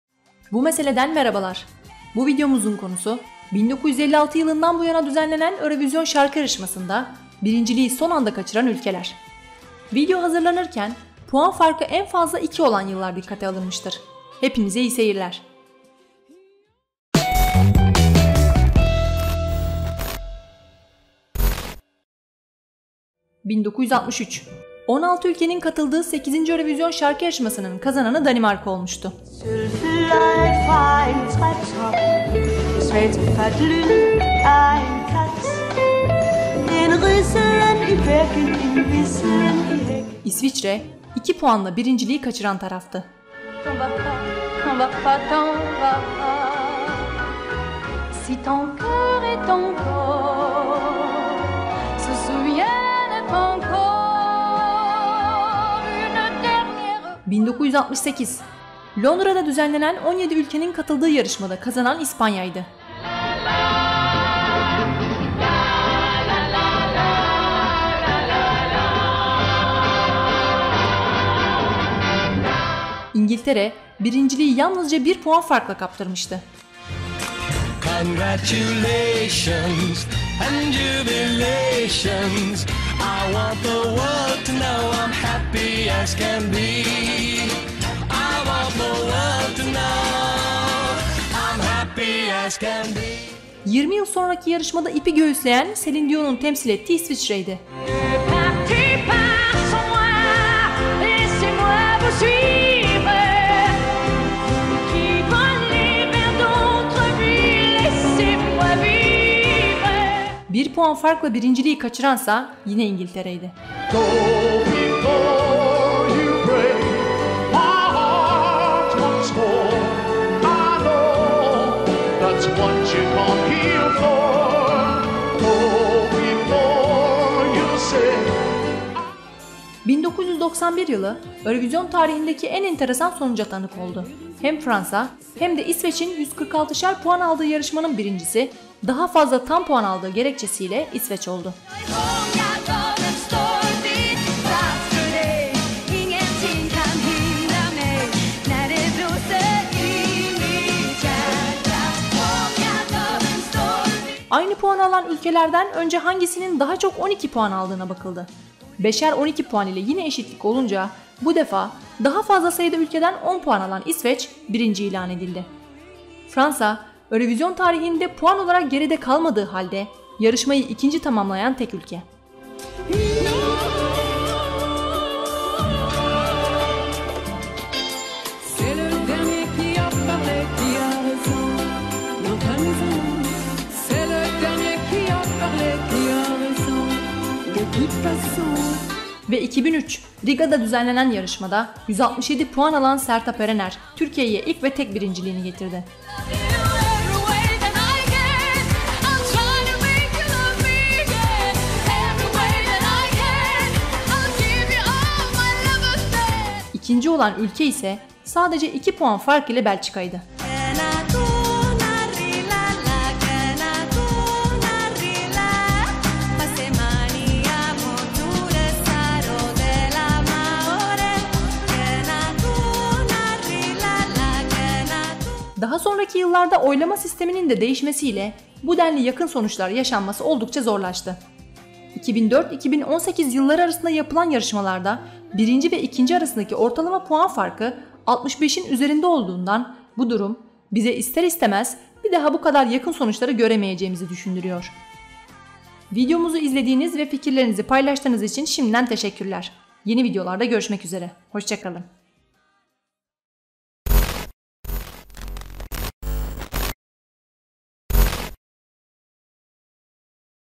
Eurovision'da Birinciliği Kıl Payı Kaçıran Ülkeler
Bu videomuzun konusu, 1956 yılından bu yana düzenlenen Eurovision Şarkı yarışmasında, birinciliği son anda kaçıran ülkeler.

Video hazırlanırken puan farkı en fazla 2 olan yıllar dikkate alınmıştır.

İyi seyirler.